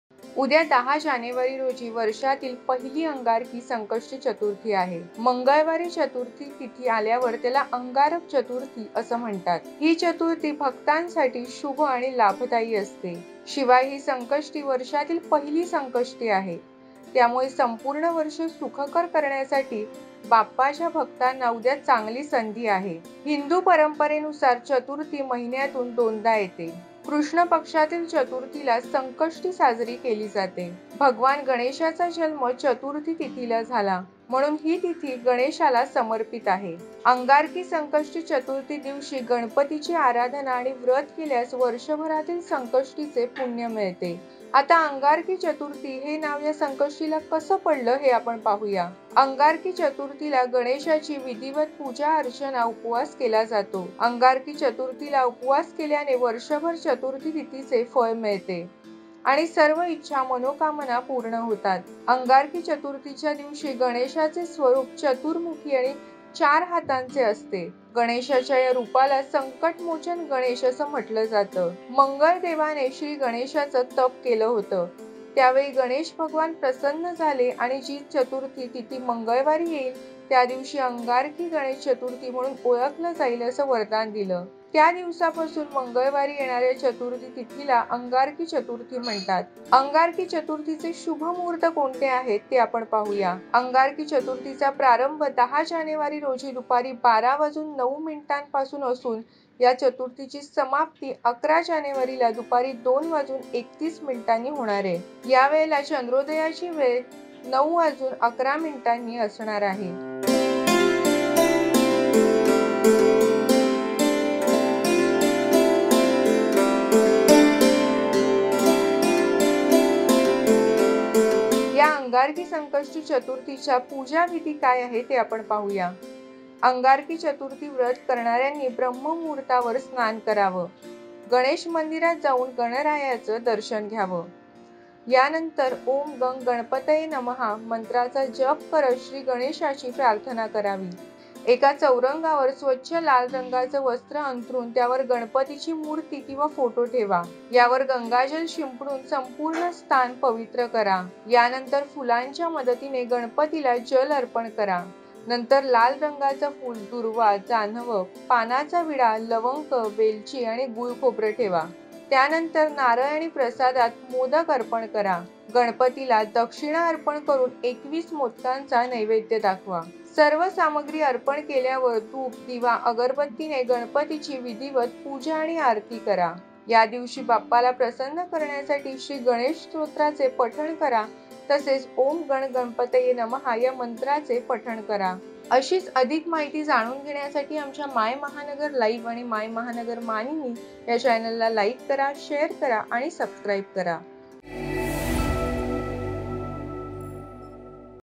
अंगारकी चतुर्थीचे जाणून घ्या शुभ मुहूर्त आणि महत्त्व
2023 ची पहिली अंगारकी चतुर्थी उद्या आहे. या अंगारकी चतुर्थीचे शुभ मुहूर्त आणि महत्त्व नक्की काय आहे आपण या व्हिडिओच्या माध्यमातून जाणून घेऊया.